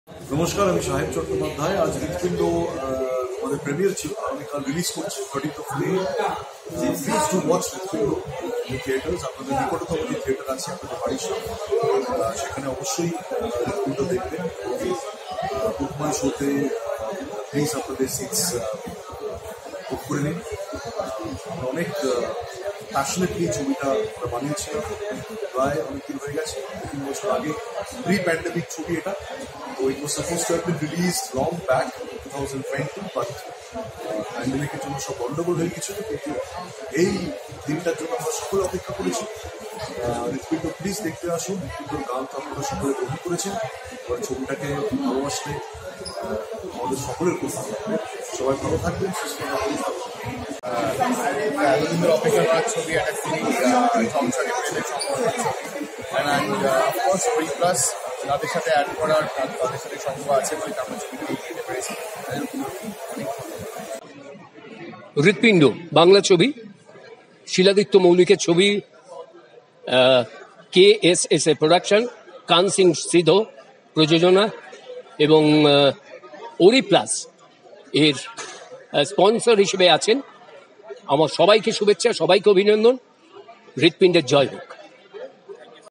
HRIDPINDO Premier | Shaheb Chattopadhyay | Prantik Banerjee | Subhasish Mukherjee | Oriplast Limited
An interview of the Tollywood Actors, ShahebChattopadhyay, PrantikBanerjee and SubhasishMukhopadhyay, talking about the new Bengali Movie, HRIDPINDO. Watch the new release Hridpindo, a touching and heart-warming love story, in your nearest theatres with your friends and family.